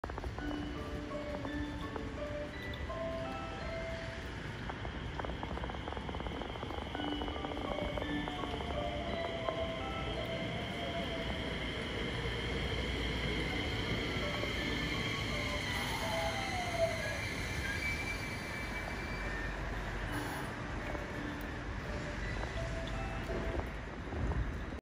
坂部線227系8番台普通緑井行き 山陽本線、可部線新白島駅到着！
広島新交通アストラムラインと接続する山陽本線新白島駅。2番のりばに到着する下関総合車両所広島支所所属227系8番台2両編成。11時31分発普通緑井行き